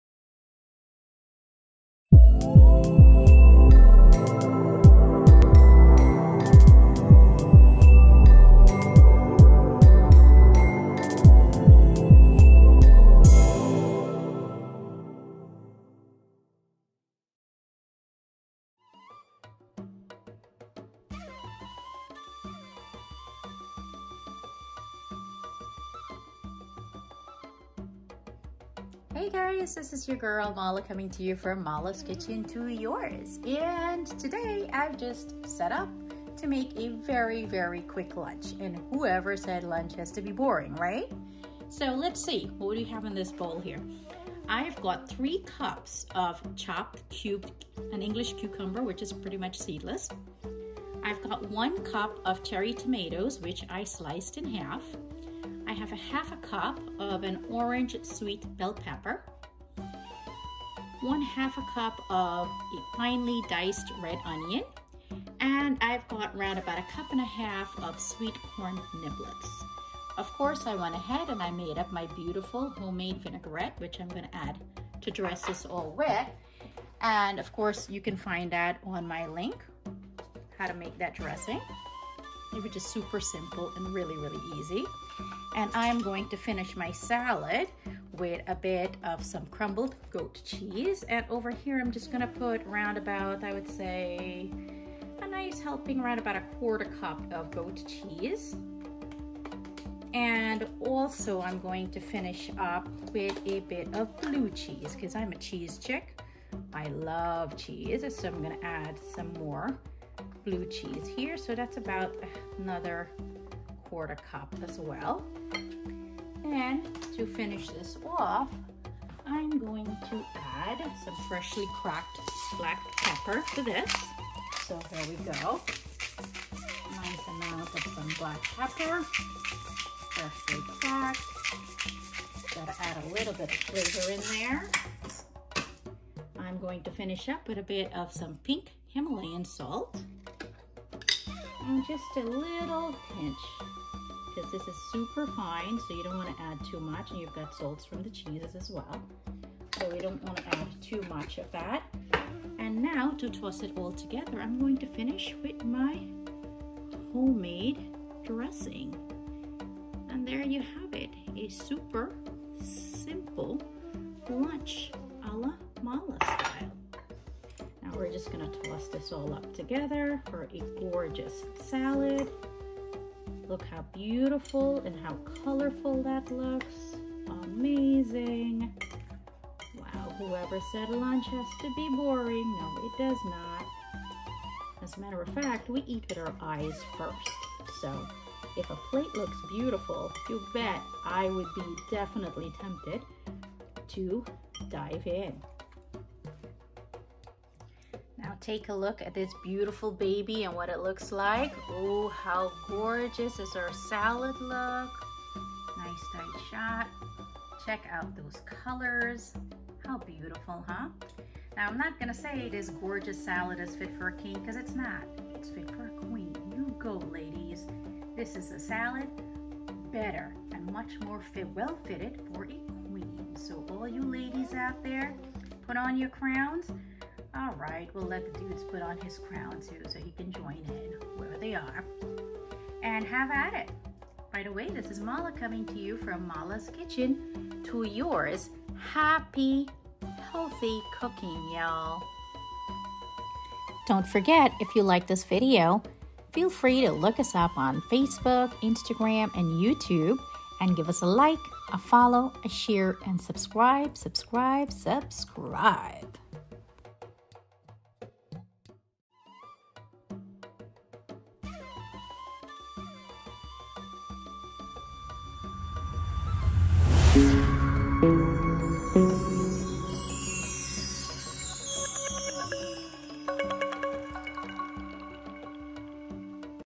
Quick Lunch~ Garden Salad
From Mala’s Kitchen To Yours...Ala Mala Style!

For Salad:

Chop any fresh veggies you have on hand.
I used:
1.5 cups swt corn niblets
3 cups chopped cukes
1 cup cherry tomatoes halved
1/2 cup finely diced red onion
1/2 cup chopped swt orange bell pepper
1/4 cup crumbled goat cheese
1/4 cup crumbled blue cheese

Add all veggies to a large bowl, add cheese, salt, pepper and toss with dressing below.

Mala’s Homemade Dressing

Ingredients
•1 lemon or lime            •Zest of 1 lemon or lime
•Juice of 1 lemon / lime  •1/4 cup white vinegar
•2 tbsp extra virgin olive oil
•1 tsp stone-ground mustard
•1/4 tsp each :  onion powder, garlic powder, black pepper, dried basil, dried oregano, dried rosemary, dried thyme                   •Salt to taste
Method
1.  In a glass jar, add all dried ingredients then the lemon or lime zest and mustard.
2. Next, all wet ingredients (citric juice, olive oil, vinegar )
3. Close jar tightly with lid and shake vigorously to combine.